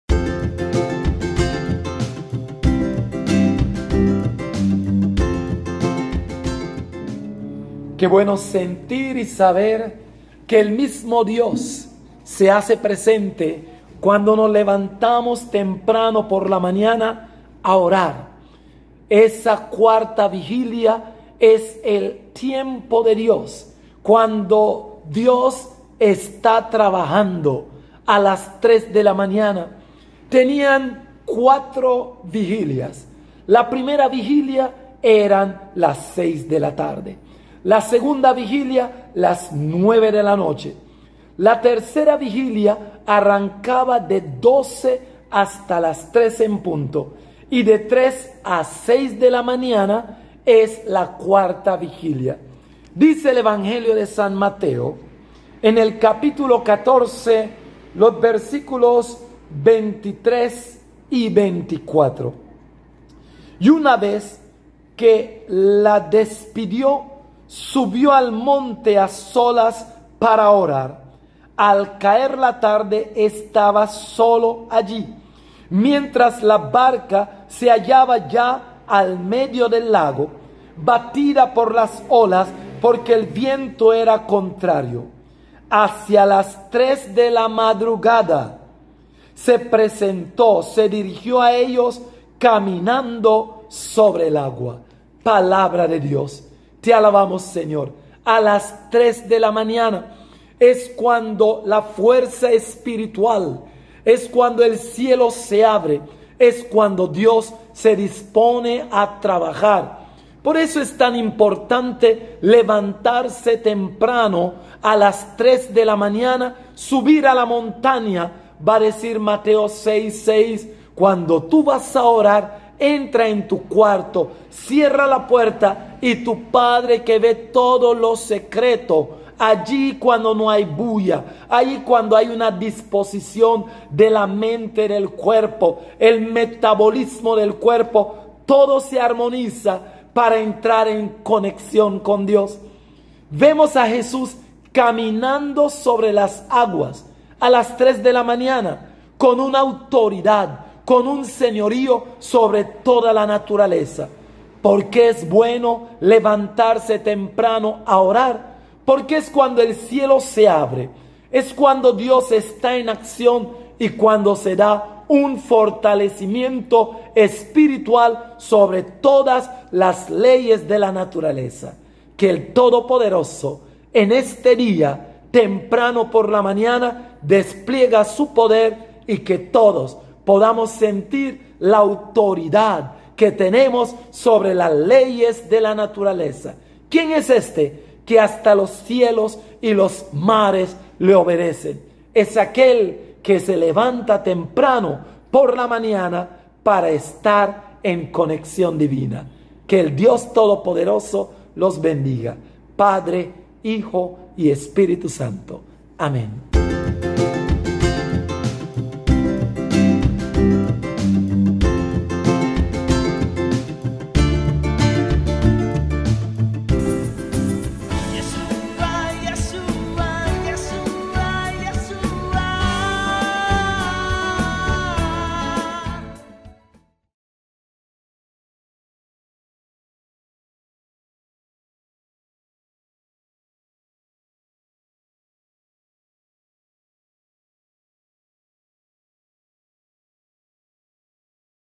Padre Marcelino Hudgson - El Poder de la Oración en la Mañana
Píldora de Vida #80 :
El Padre Marcelino Hudgson nos habla del Poder de la oración